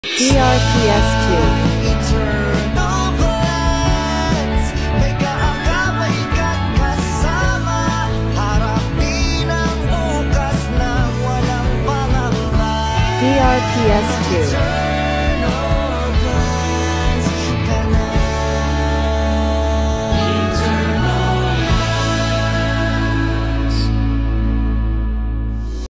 Eternal Plans Radio Ad 2023 26s
Ito po ang 2023 Radio Ad ng Eternal Plans, ang Insurance company mula sa ALC Group of Companies.

DRPS Two 3150: Eternal Plans Radio Ad 2023 26s